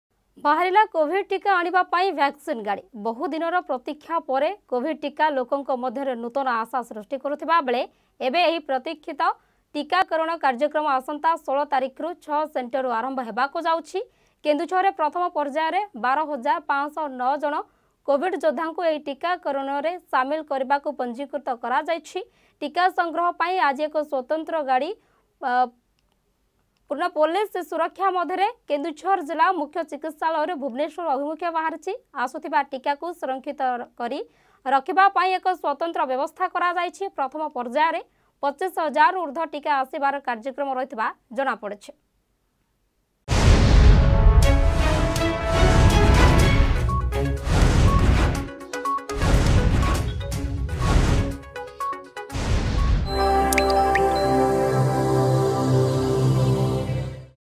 ବାହାରିଲା କୋଭିଡ ଟୀକା ଆଣିବା ପାଇଁ ଭ୍ୟାକ୍ସିନ ଗାଡି ....
ବହୁ ଦିନର ପ୍ରତୀକ୍ଷା ପରେ କୋଭିଡ ଟୀକା ଲୋକଙ୍କ ମଧ୍ୟରେ ନୂତନ ଆଶା ସୃଷ୍ଟି କରିଥିବା ବେଳେ ଏବେ ଏହି ପ୍ରତୀକ୍ଷିତ ଟୀକା କାରଣ କାର୍ଯ୍ୟକ୍ରମ ଆସନ୍ତା 16 ତାରିଖରୁ 6 ସେଣ୍ଟର ରୁ ଆରମ୍ଭ ହେବାକୁ ଯାଉଛି ...